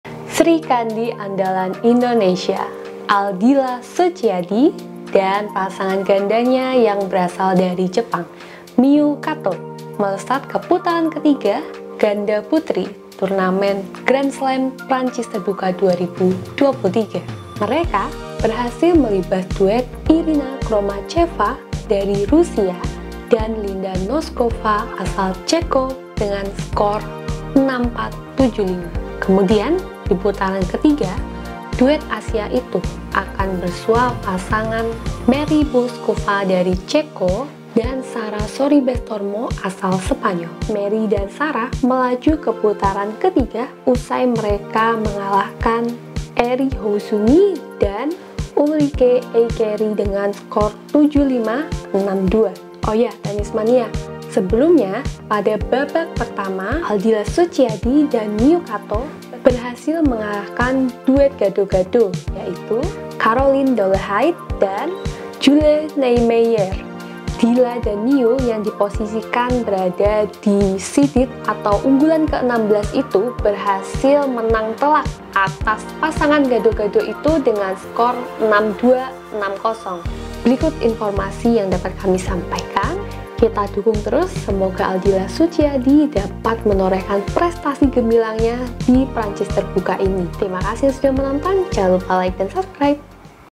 ALDILA SUTJIADI / MIYU KATO ADVANCES TO THE 3RD ROUND OF THE ROLAND GARROS | GRAND SLAM
Petenis andalan Indonesia, Aldila Sutjiadi, yang berpasangan dengan petenis Jepang, Miyu Kato, melaju ke putaran ketiga ganda putri turnamen tenis Grand Slam Prancis Terbuka 2023, setelah mereka melibas duet Irina Khromacheva dari Rusia dan Linda Noskova asal Ceko, straight set 6-4, 7-5. Berikutnya di putaran ketiga, duet Asia itu akan bersua pasangan Marie Bouzkova dari Ceko dan Sara Sorribes Tormo asal Spanyol, yang melangkah ke putaran ketiga usai mendepak Eri Hozumi dan Ulrikke Eikeri, 7-5, 6-2

Eng:
Indonesian mainstay tennis player, Aldila Sutjiadi, who is partnered with Japanese tennis player, Miyu Kato, advanced to the third round of the women's doubles tennis tournament at the 2023 French Open Grand Slam, after they beat the duo Irina Khromacheva from Russia and Linda Noskova from Czech, straight sets 6-4, 7-5. Next in the third round, the Asian duo will meet Marie Bouzkova from the Czech Republic and Sara Sorribes Tormo from Spain, who advanced to the third round after beating Eri Hozumi and Ulrikke Eikeri, 7-5, 6-2